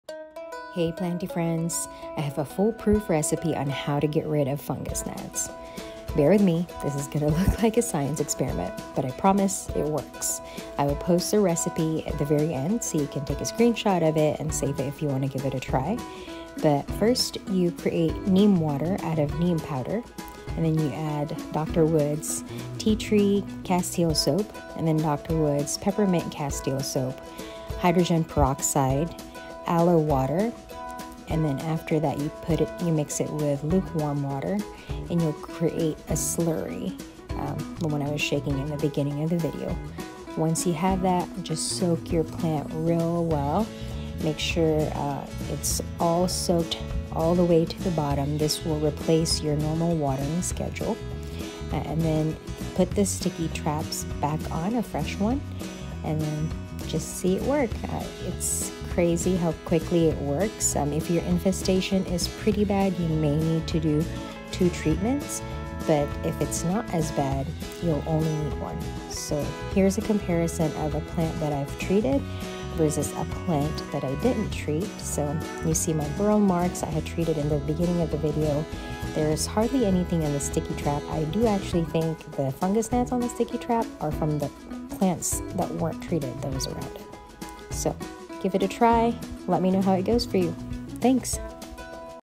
How to get rid of fungus gnats for good
This is not the cheapest way to get rid of fungus gnats but it’s by far the most effective I’ve tried. The ingredients do go a long way so you can treat a lot of plants. Just make sure to treat them all at once to fully stop the spread.

What you’ll need:

- 2 Tbsp Neem Powder
- 2 Cups water for the neem water solution
- ¼ Cup Dr Woods Tea Tree Castile Soap
- ¼ Cup Dr Woods Peppermint Castile Soap
- ½ Cup Pure Aloe Vera Juice
- 1 Tbsp 3% Hydrogen Peroxide
- 4 Cups lukewarm water

How to make neem water:

- Mix 2 tbsp of neem powder with 2 cups of water
- Let the solution sit for at least an hour
- Transfer the water to another container through a strainer
- Mix the liquid with the rest of the ingredients above